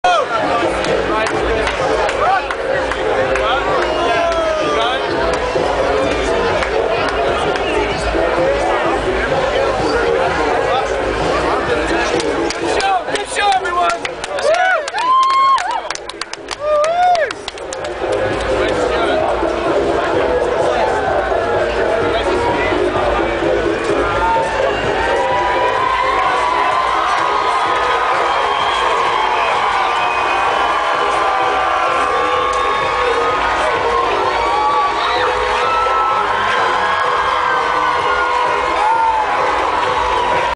Rhymefest, Kanye West & Mark Ronson Backstage @ Lollapalooza
Rhymefest, Kanye West, Mark Ronson, Pete Wentz & Ashley Simpson backstage at Lollapalooza 2008.